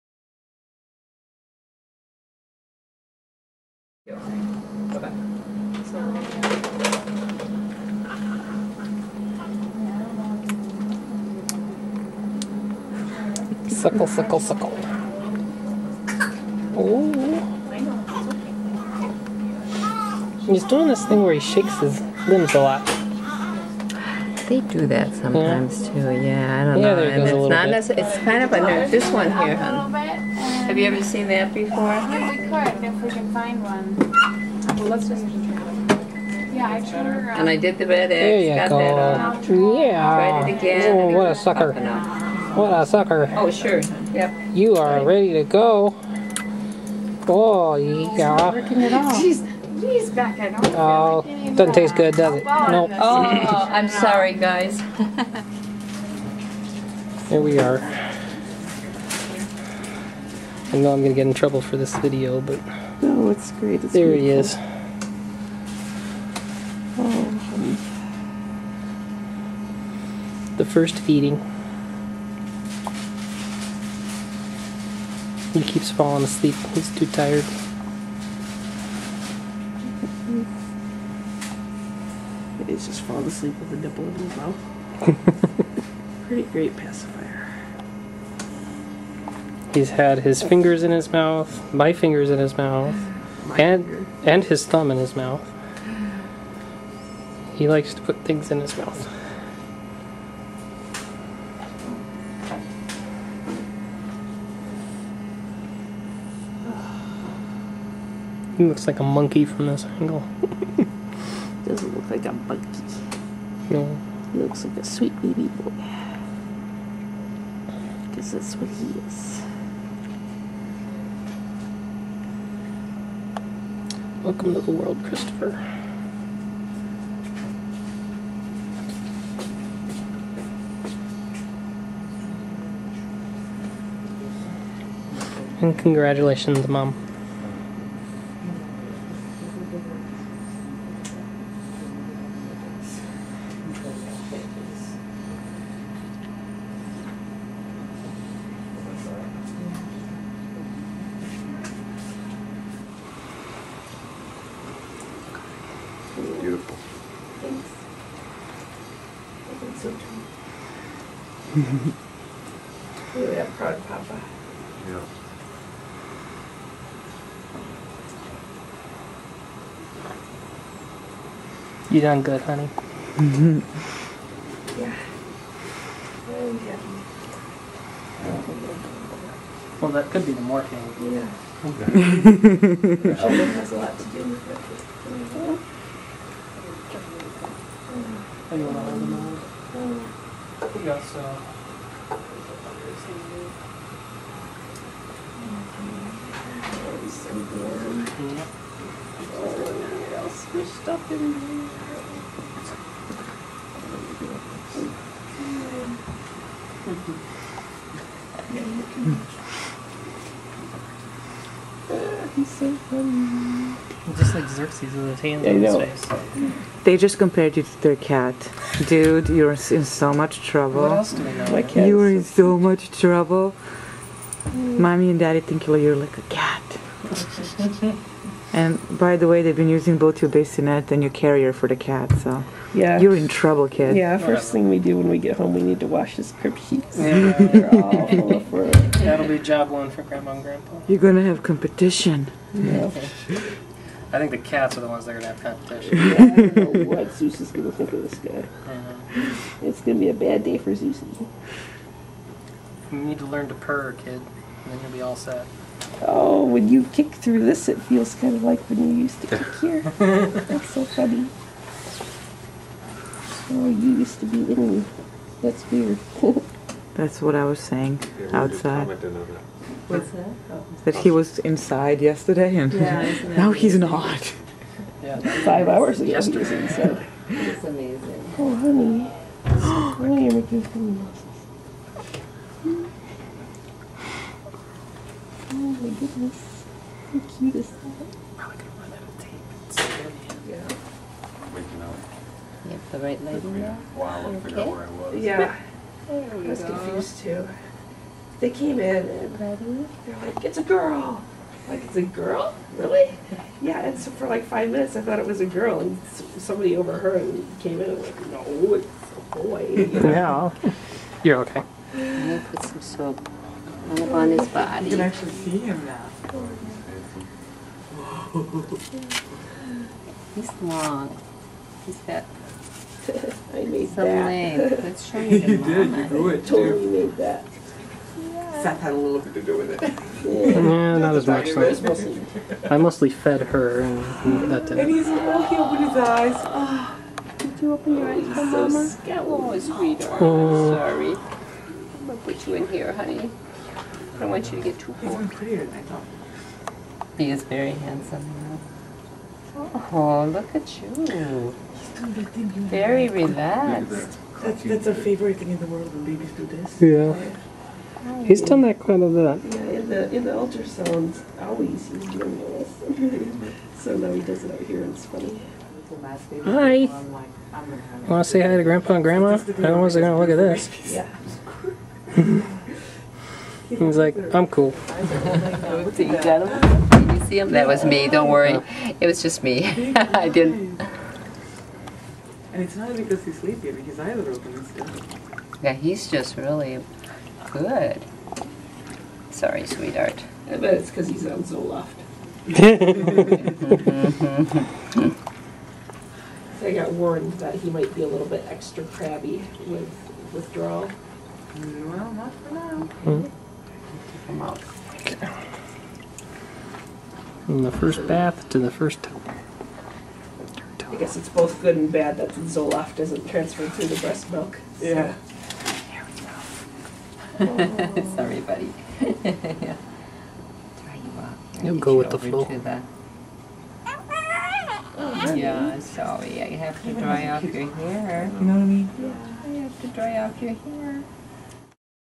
A New Day (Part II)
Our new son Christopher right after birth.  His first four days on earth (spent in the hospital).